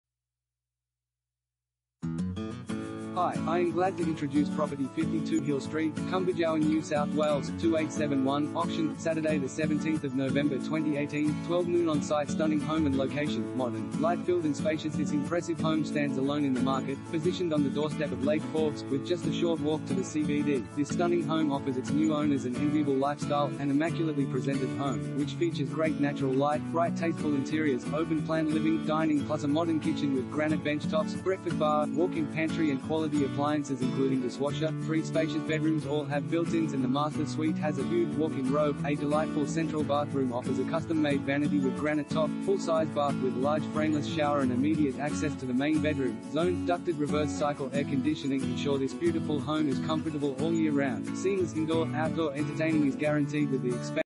52 Hill Street, Cumbijowa NSW 2871 | AuPropertyReport.Com
52 Hill Street, Cumbijowa NSW 2871
Property Type: house
Bedrooms: 3
Bathrooms: 3
Car Space: 2
AUCTION - Saturday 17th November 2018